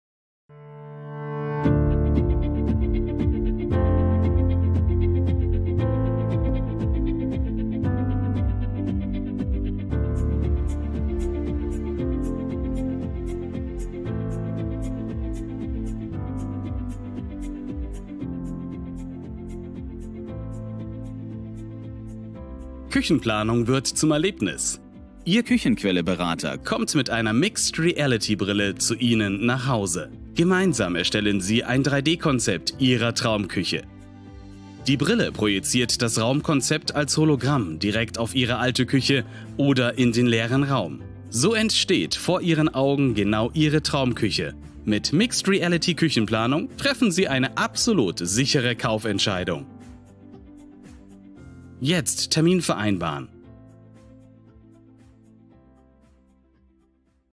Die küchenquelle Zuhauseplanung mit Mixed Reality
Noch vor der Kaufentscheidung die Küche zuhause erleben: Die MR-Brille projiziert ein Hologramm der geplanten Küche in den Raum. Änderungen können in Echtzeit am Tablet vorgenommen und in die MR-Brille eingespielt werden. So sieht man genau, wie sich der neue Küchen(t)raum ins Zuhause einfügt.